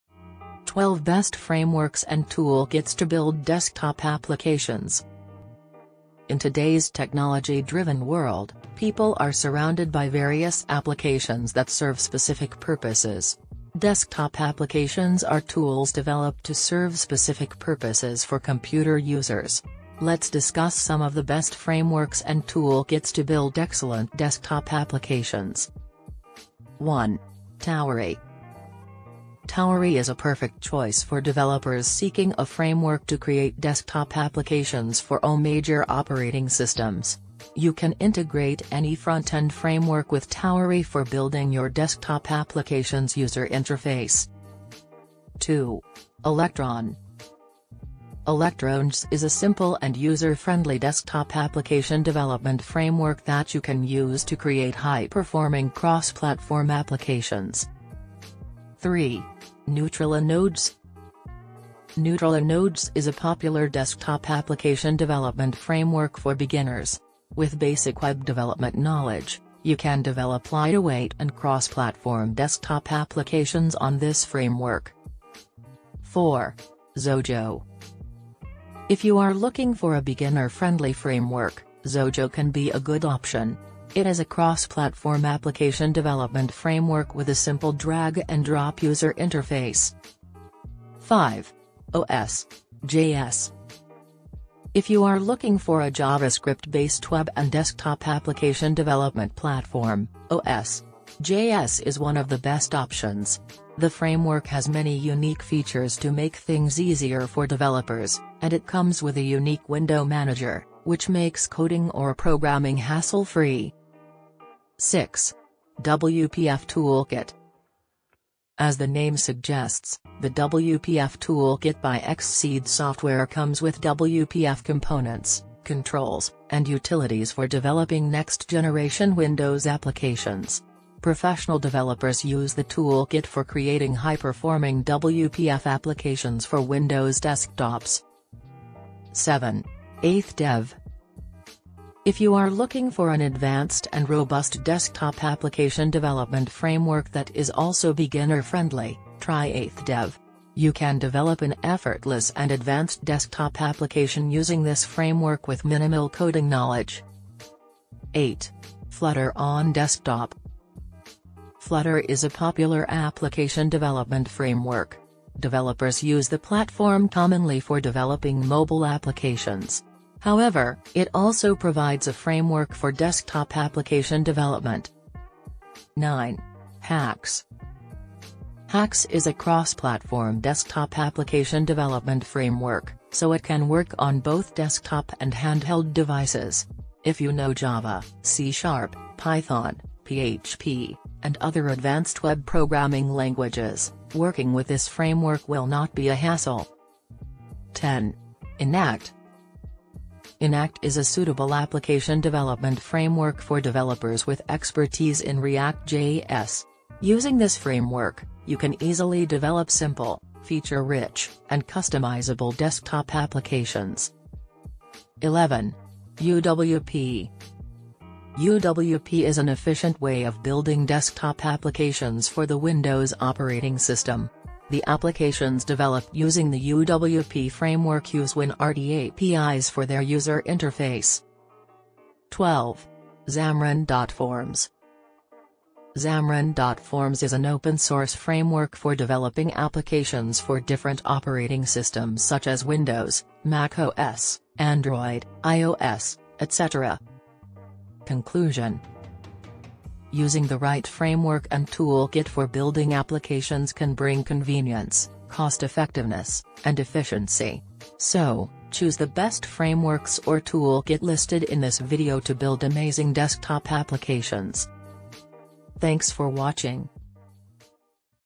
Best Frameworks and Toolkits to Build Desktop Applications
1.     Tauri
2.     Electron
3.     Neutralinojs
4.     Xojo
5.     OS.js
6.     WPF Toolkit
7.     8th Dev
8.     Flutter on Desktop
9.     Haxe
10.   8th Dev
11.   Flutter on Desktop
12.   Haxe